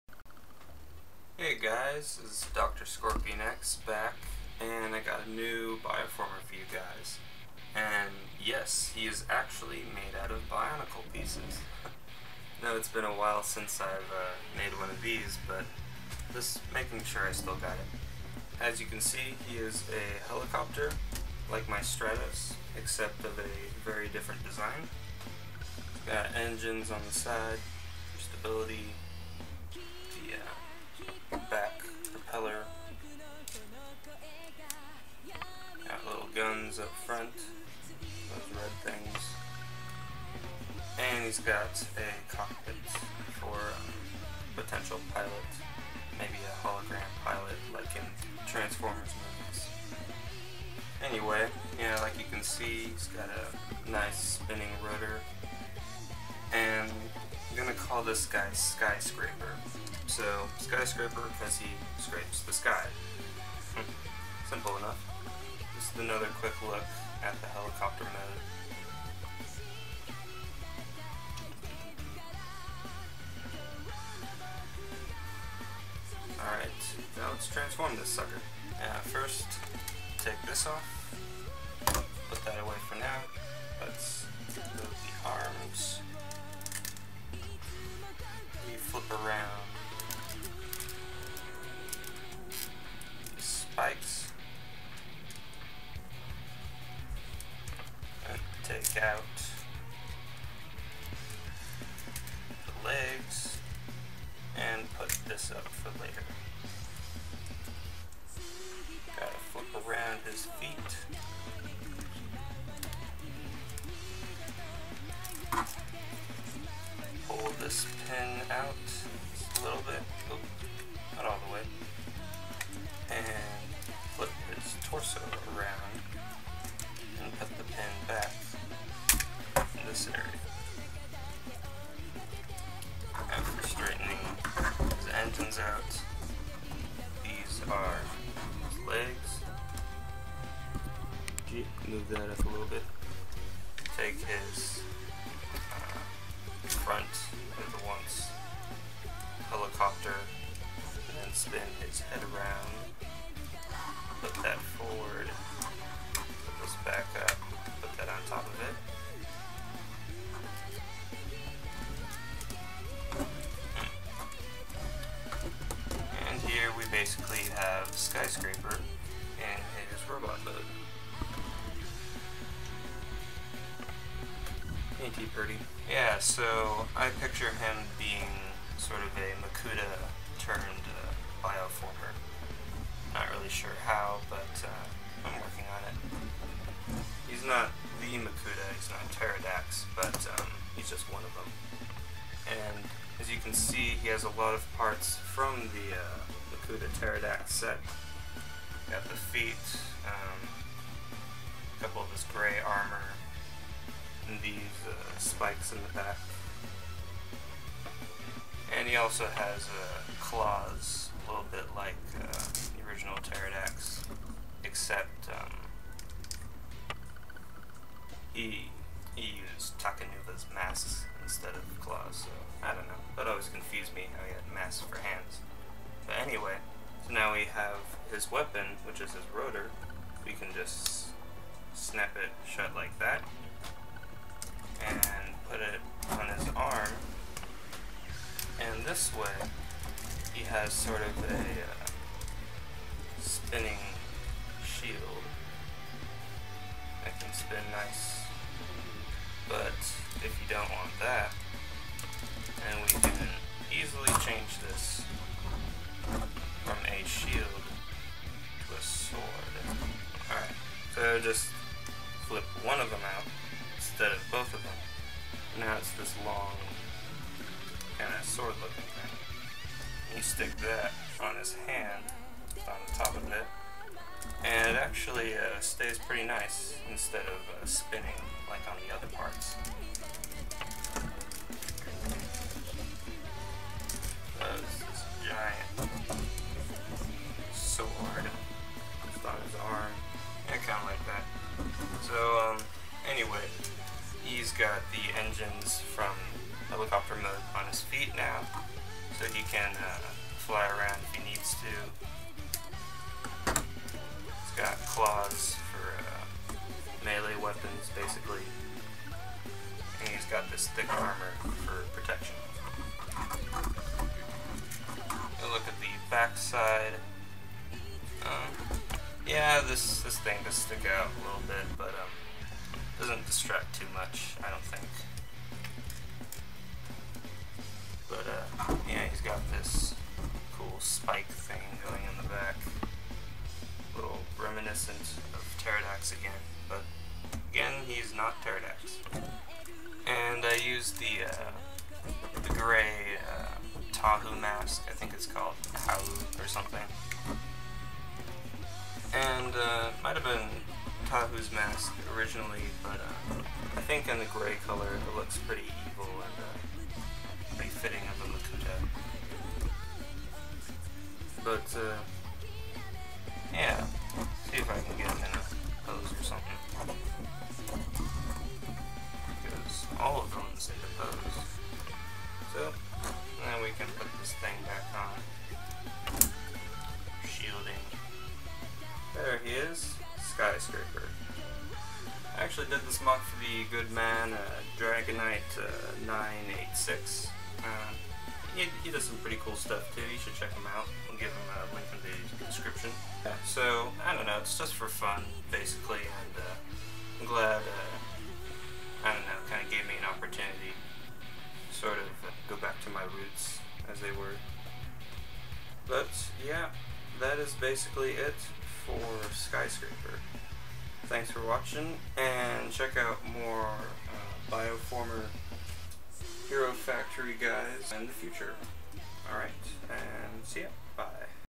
Bioformer Reviews: Skyscraper (Dragonite transformer lego moc contest)
A bionicle transformer! With razor claws, jet engines, and a massive sword/shield, this makuta is ready to tear it up!